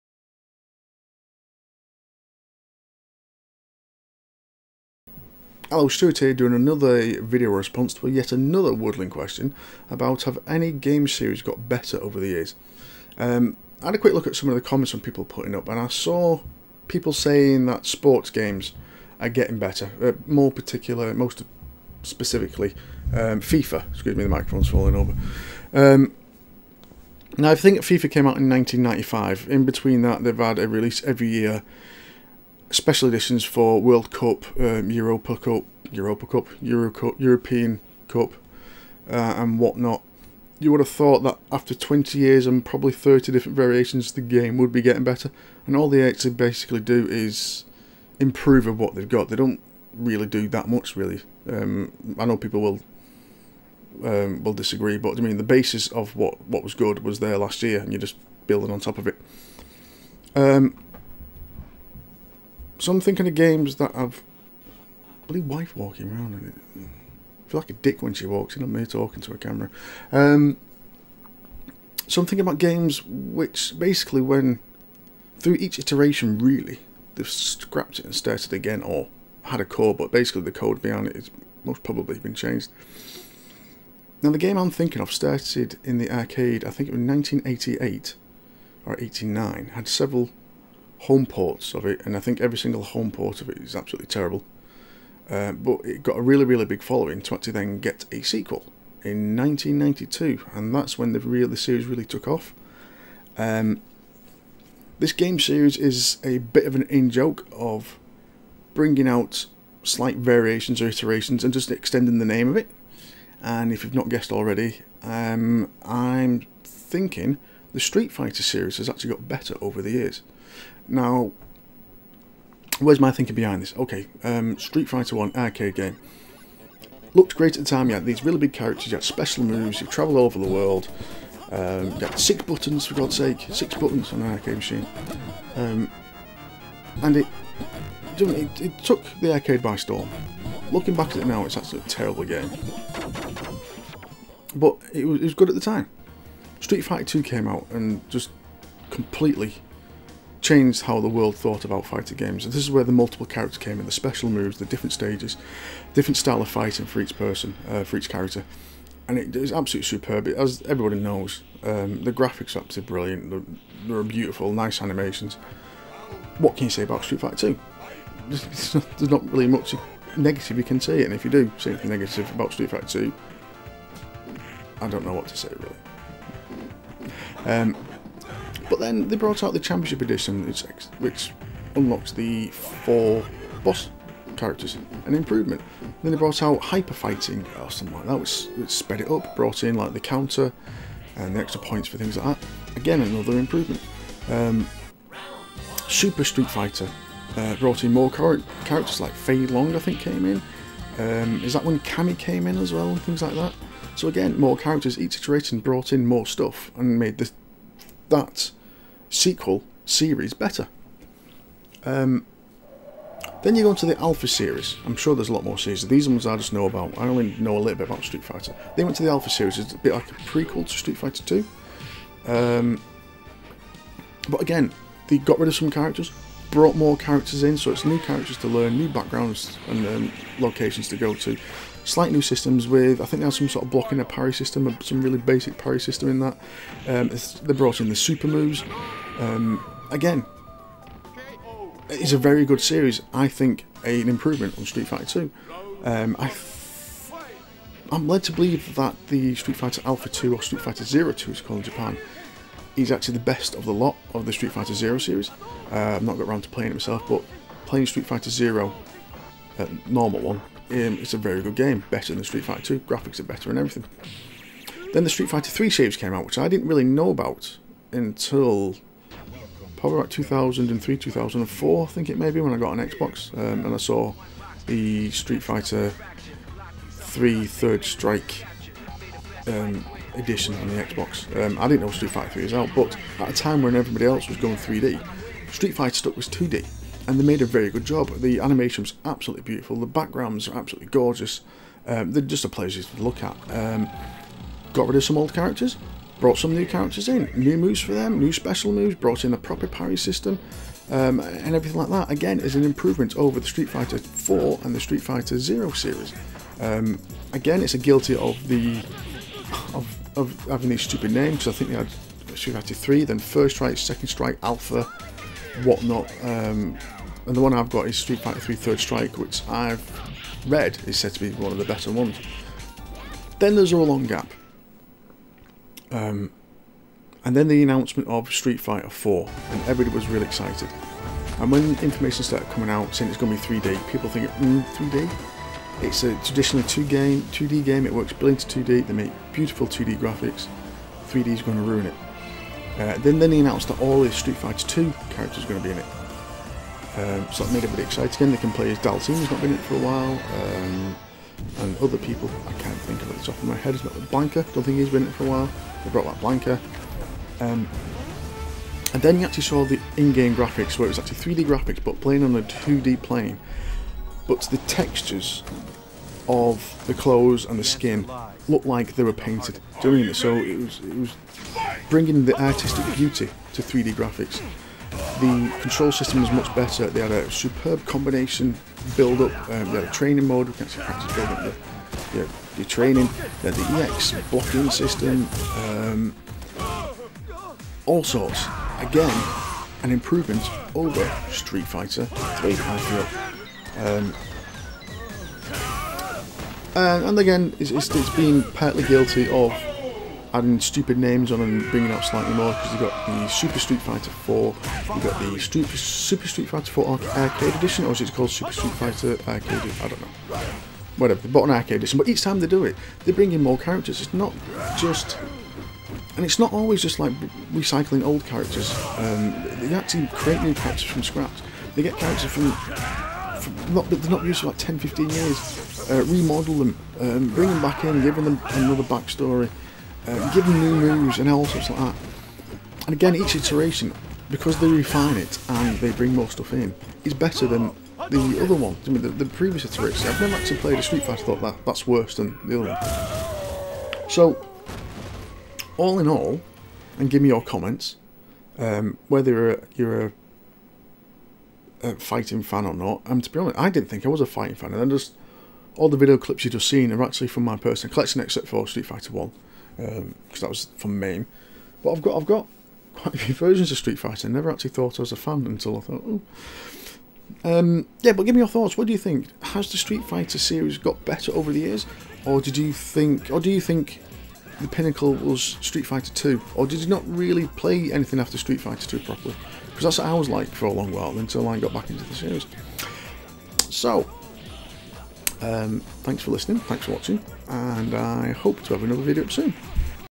VR: Woodlin, are any game series getting better or are they all getting worse
I am no expert on this subject and some of my comments aren't entirely accurate, but the general idea is there